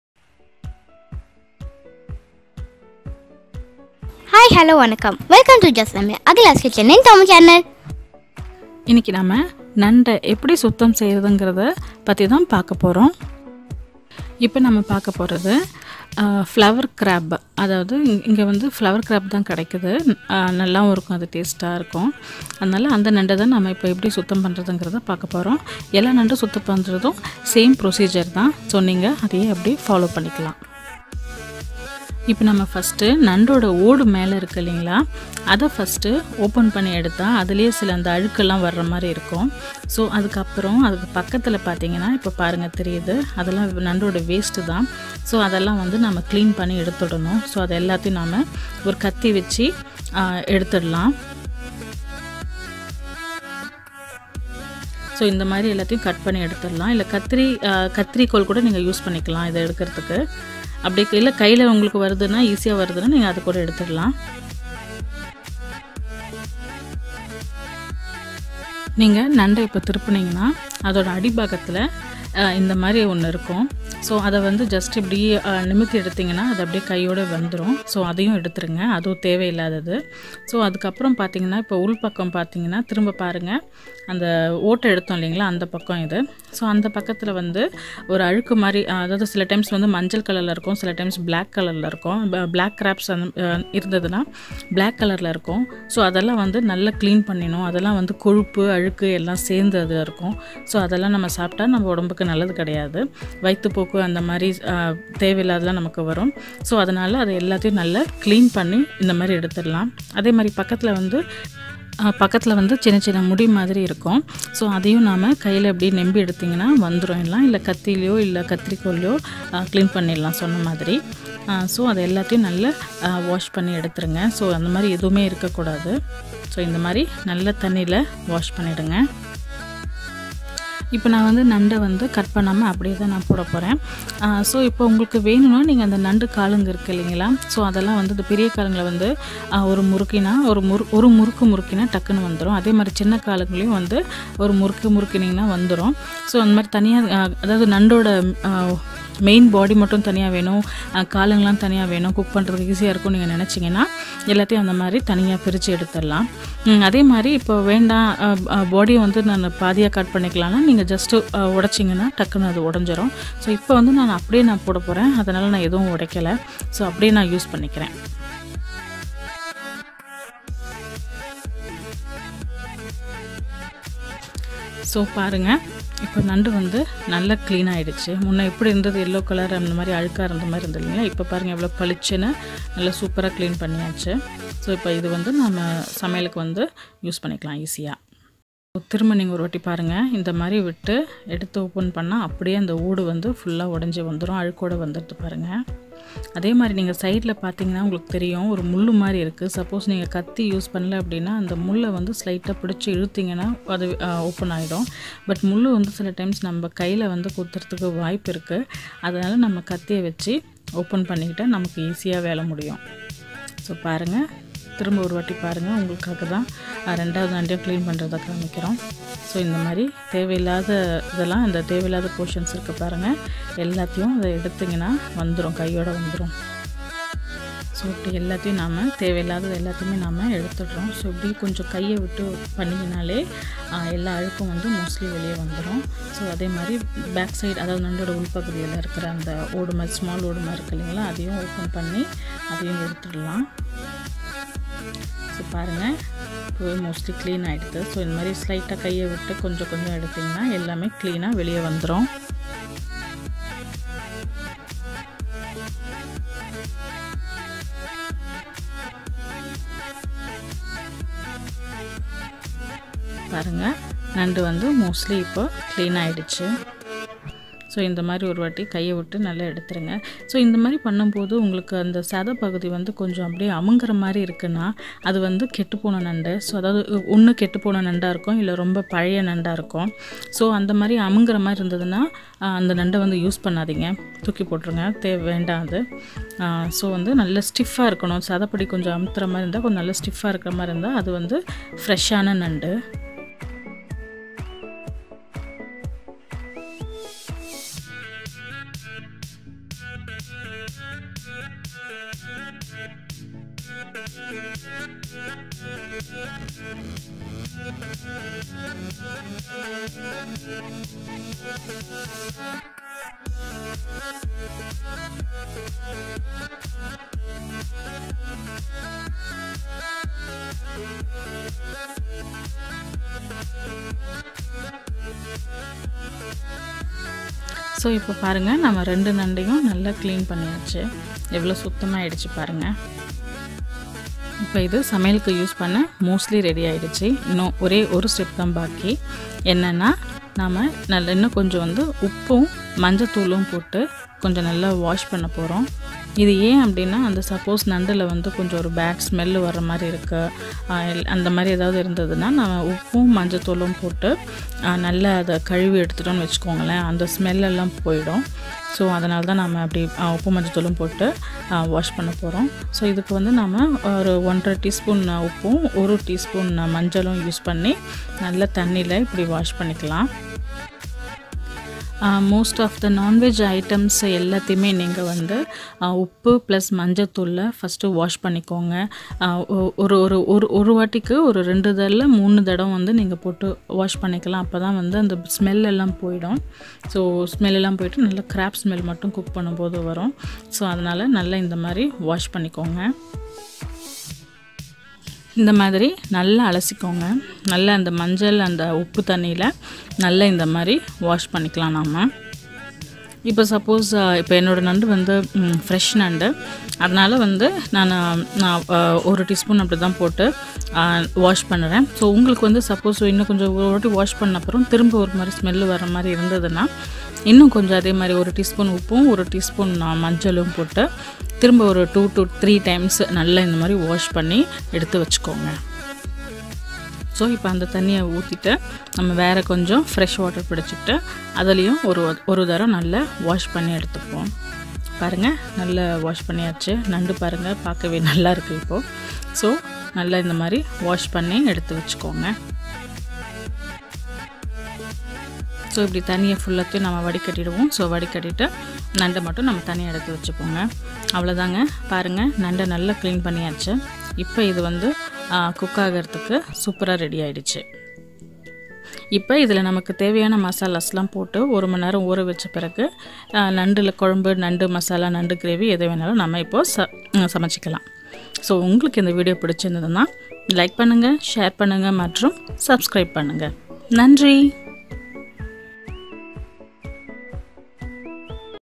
How to clean Nandu in Tamil | How to clean crab in Tamil
How to clean Nandu in Tamil

Many people think, cleaning nandu is a tedious task. But once you learn it, it is very easy.

Here with the help of my father, we show how to clean the nandu.

Happy cooking.